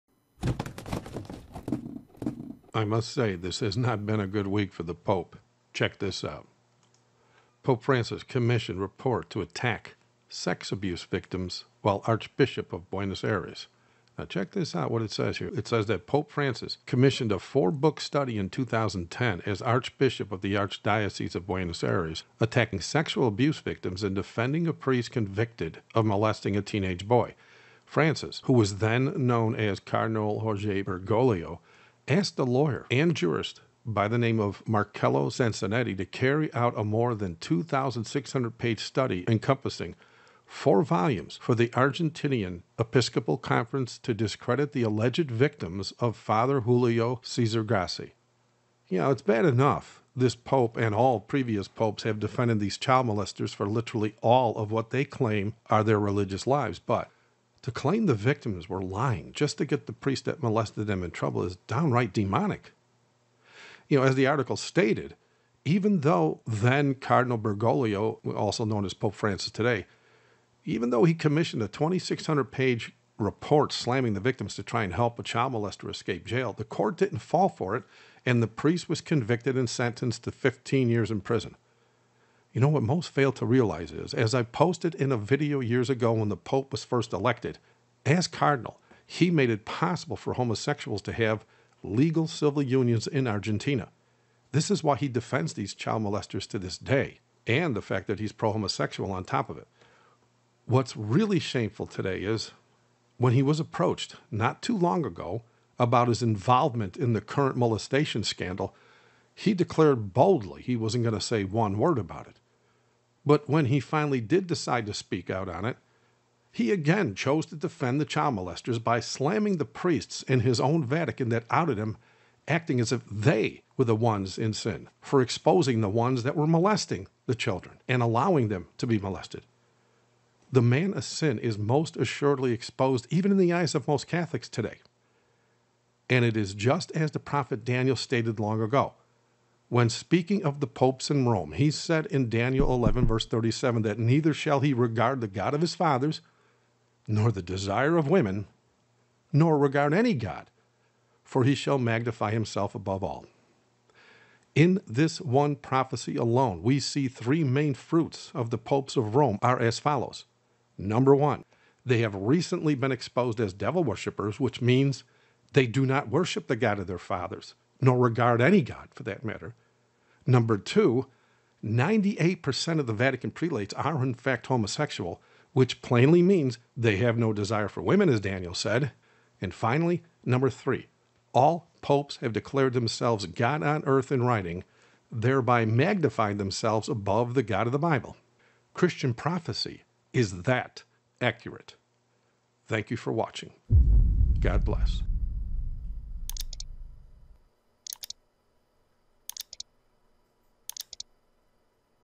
Pope Attacked Abuse Victims!
Turns out, this Pope's desire to protect child molestors and attack their victims has been the norm for him even before he became Pope!

All links to the videos and articles mentioned in this video are in my poGm blog entry